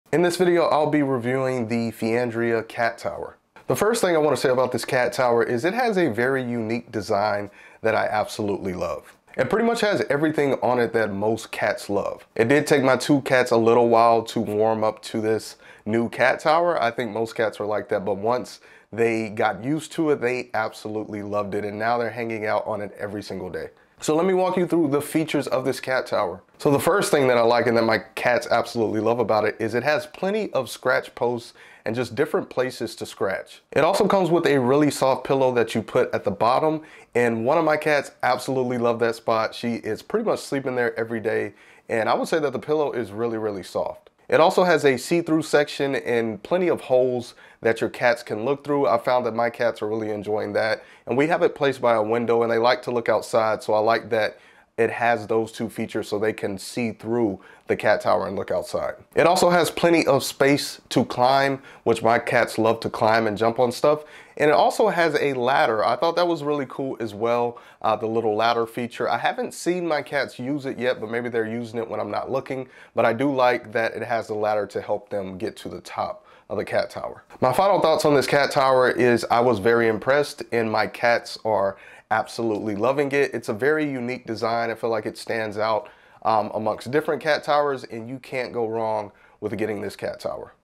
I LOVE This Cat Tower | Amazon Finds For Cats!
My honest review of the Feandrea Clickat Collection Cat Tree!